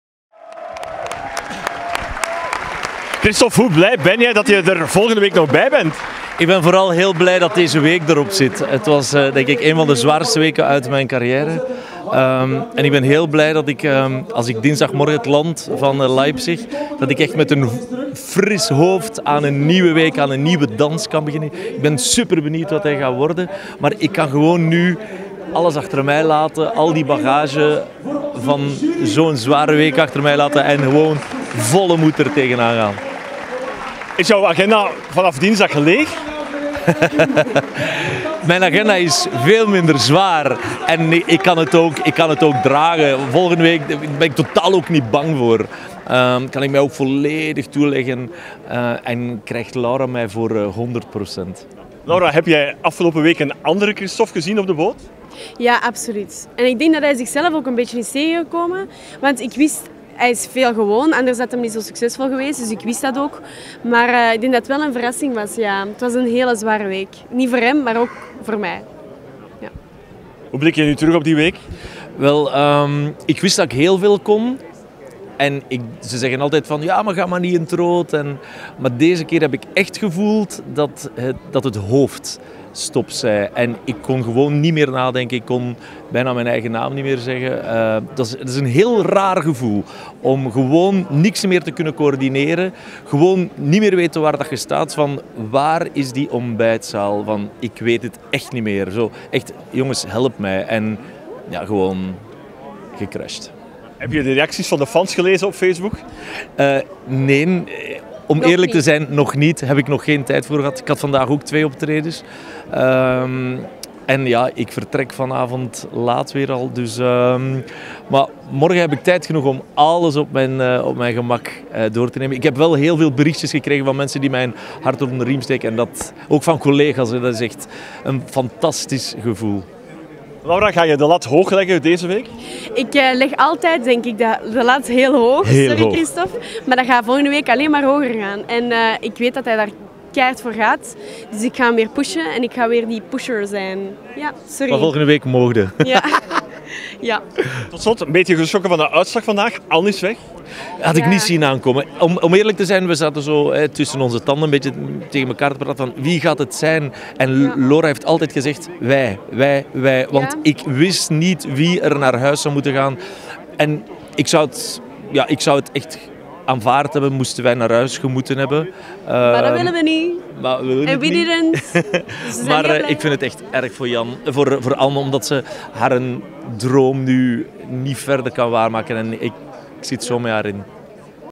Christoff na crash: "Ik kon niet meer nadenken"
Christoff had geen makkelijke week achter de rug en dat was er zondagavond aan te zien: "Het was één van de zwaarste weken uit mijn carrière", vertelde hij aan onze reporter. Zondag staat hij weer vol vertrouwen op de dansvloer bij Dancing With The Stars